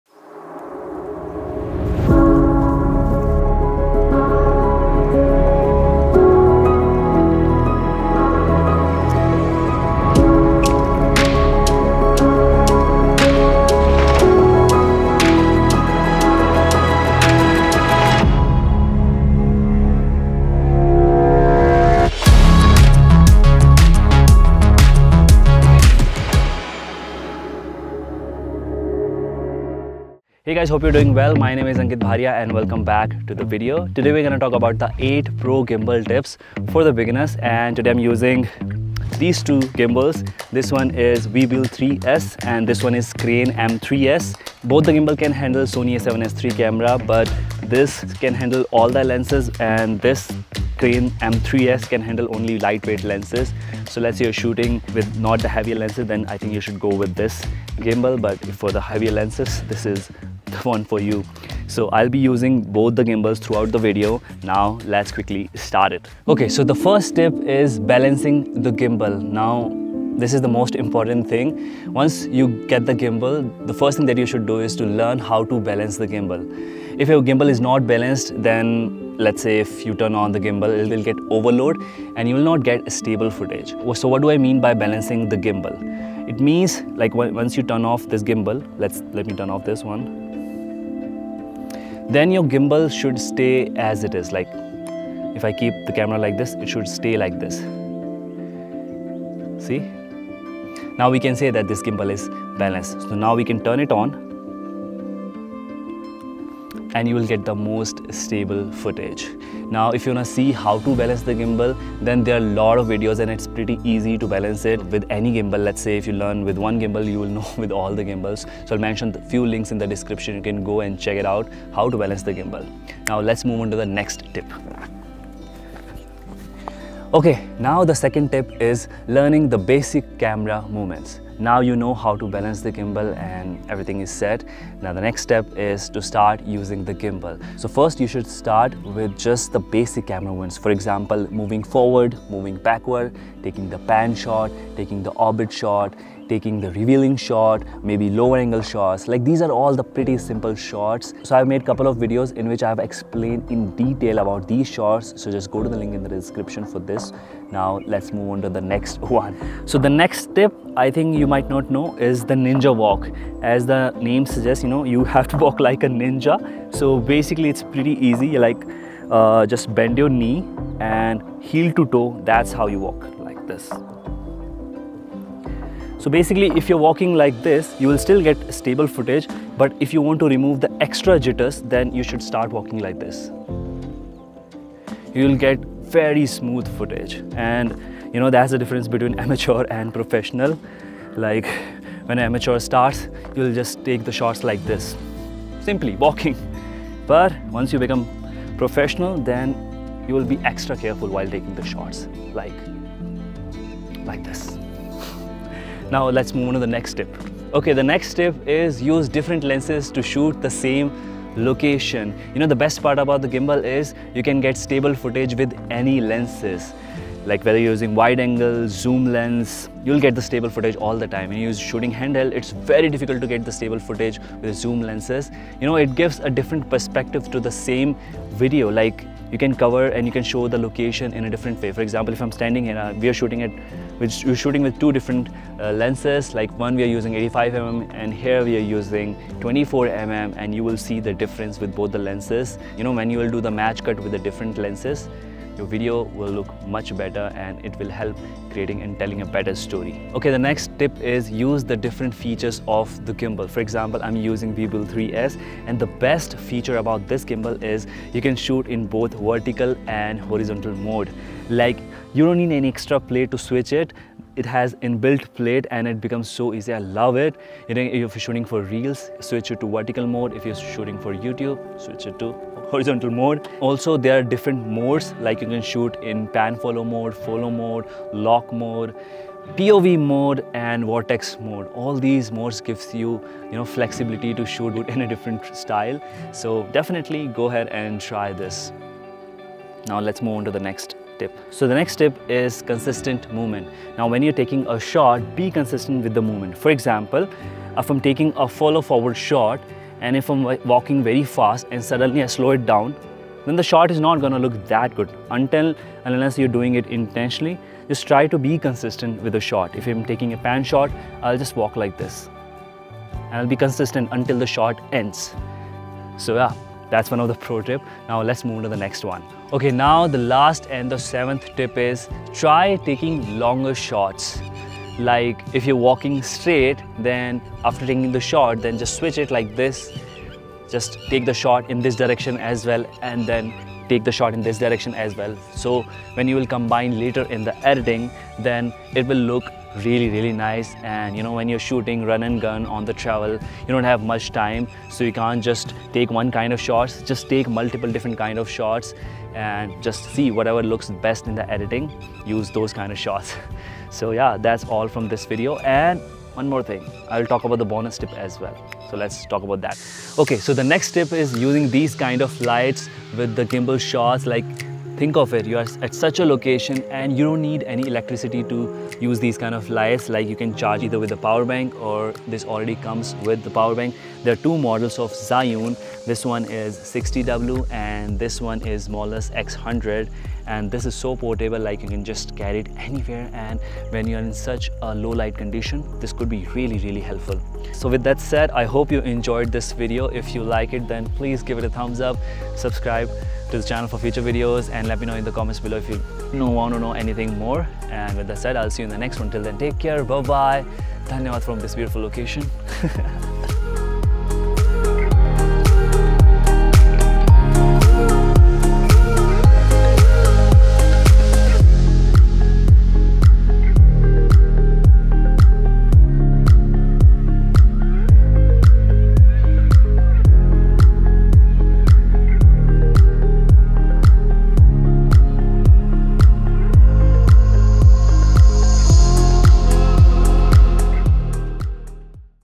8 Camera Gimbal Tricks - Cinematic
Talking about 8 Pro Camera Gimbal Tips for Beginners.

0:00 INTRO
1:05 BALANCING THE GIMBAL
2:08 CAMERA MOVEMENTS
2:42 NINJA WALK
3:28 USE DIFFERENT LENSES
4:12 USE FEATURES OF YOUR GIMBAL
4:53 CONSISTENT MOVEMENT
5:22 TAKE LONGER SHOTS
6:00 PORTABLE LIGHTS

Thanks a lot @ lalitchoudharyy for helping me out with the shoot.